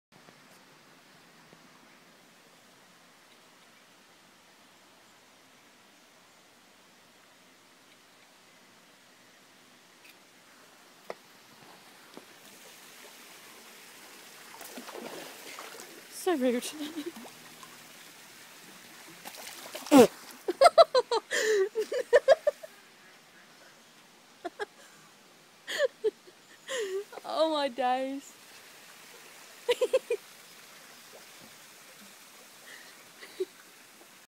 Cheeky penguins
Penguins showing off at birdland in uk (GL54 2BN)